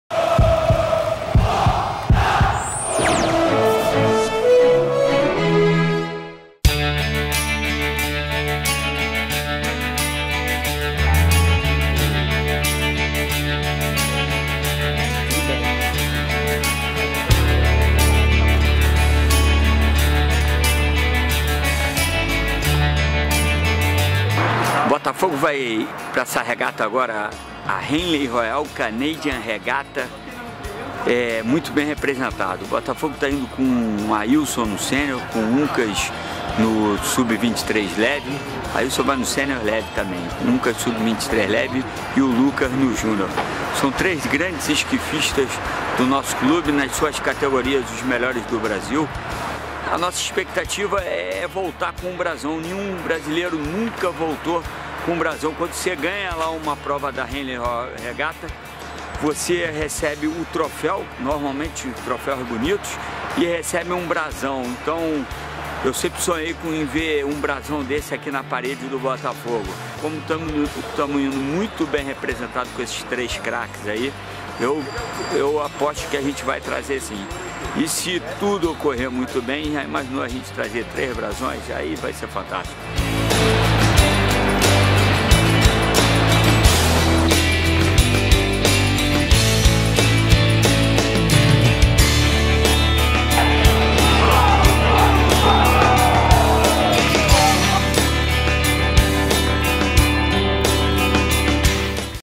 BOTAFOGO VIAJA PARA A ROYAL CANADIAN HENLEY REGATTA 2016
Boa sorte ao nossos atletas Ailson Eraclito, Lucas Verthein e Uncas Tales que viajam amanhã (28/07) para competirem pelo nosso BOTAFOGO na Royal Canadian Henley Regatta em Toronto, Canadá. Representem, estaremos na torcida!
Marcelo Murad, diretor